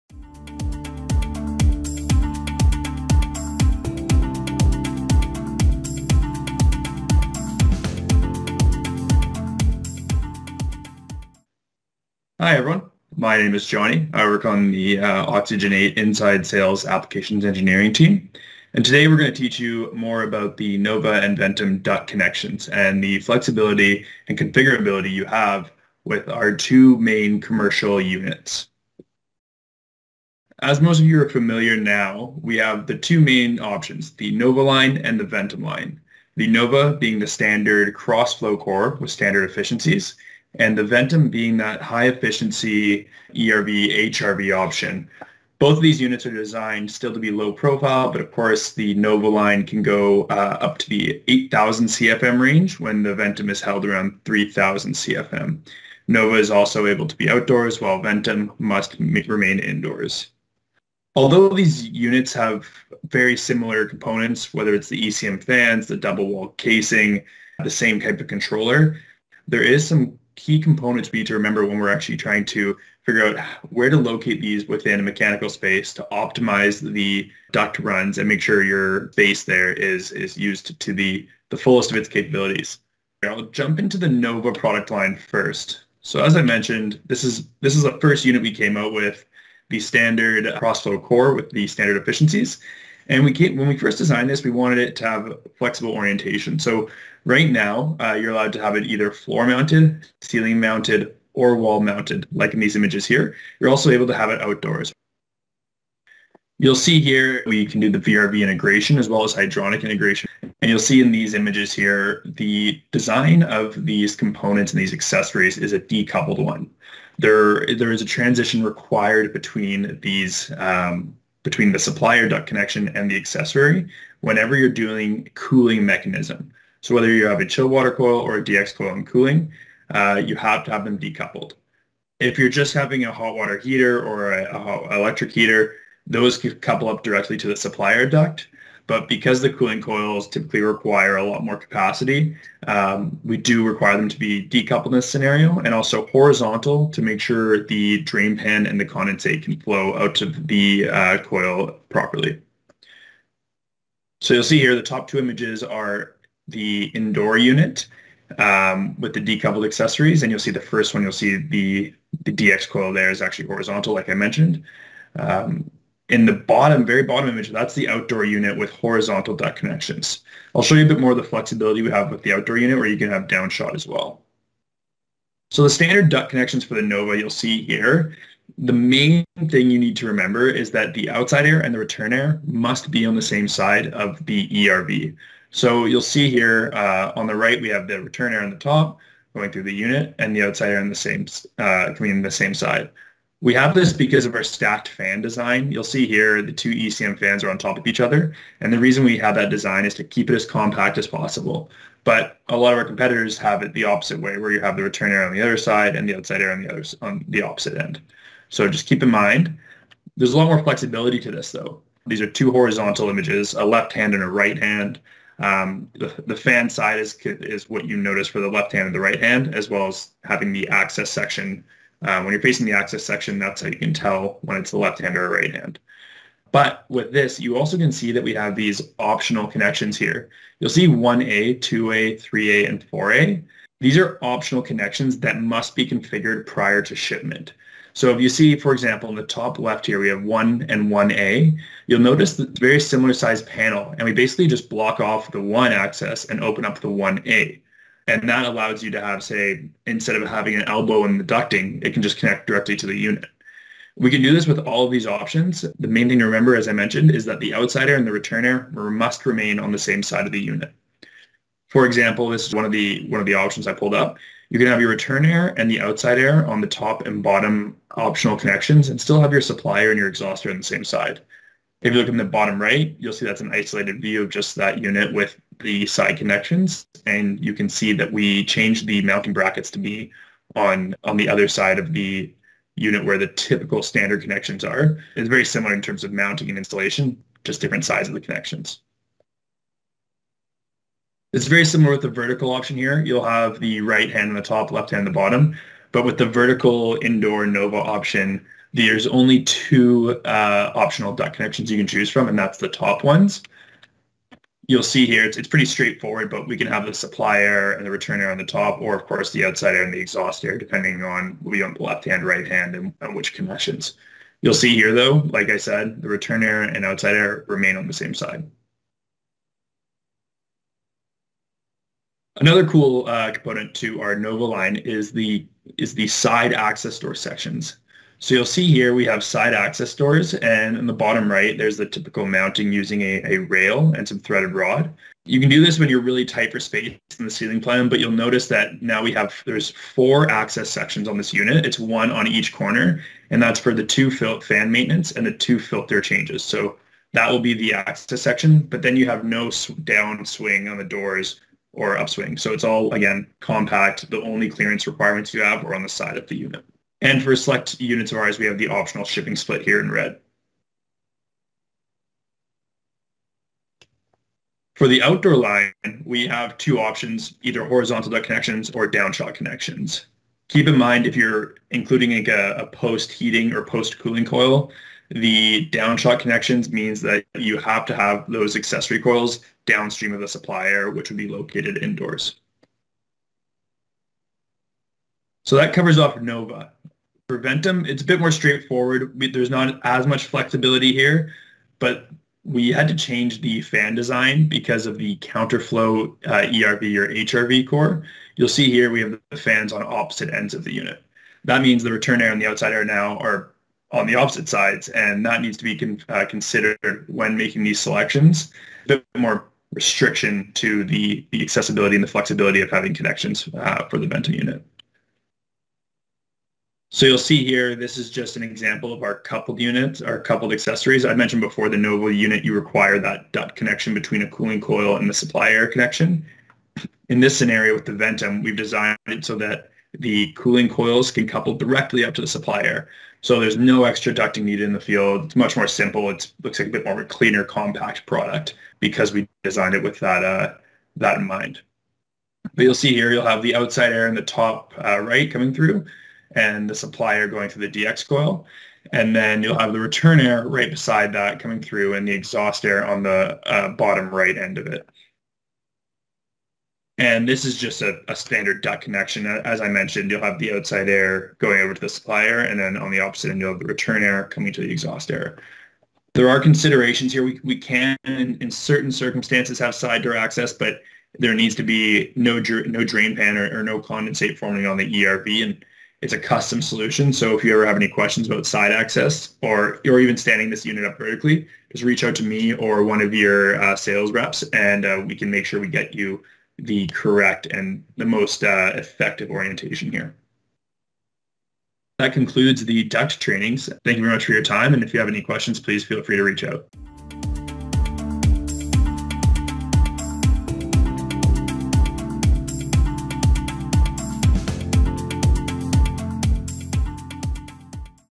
8-Minute Duct Connections for Nova and Ventum | Oxygen8
Learn about the various duct connection options for Oxygen8's commercial ventilation solutions, Nova and Ventum.

-- Timestamps --
0:00 Introduction
0:30 Nova and Ventum Overview
1:25 Nova
3:00 Standard Nova Connections
3:30 Flexible Nova Connections
6:04 Nova Outdoor Connections
6:25 Ventum
7:45 Ventum Connections